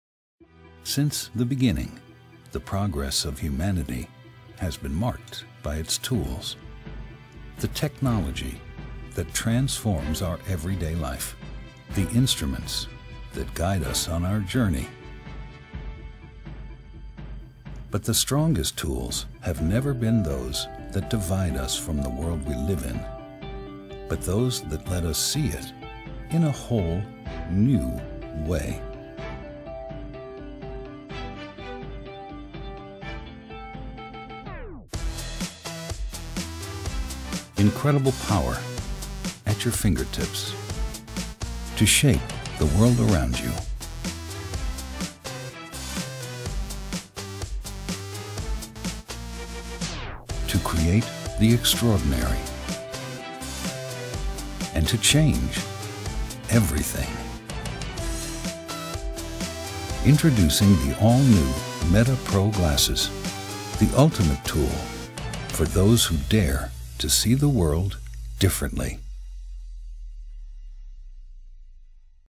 MetaPro Computer Augmented Reality Glasses
Turn your ideas into things. The power of creation at your finger tips.

Meta Pro Pocket Computer brings the most powerful pocket processing to the most powerful augmented reality glasses, enabling you to leap into the final form factor of computing.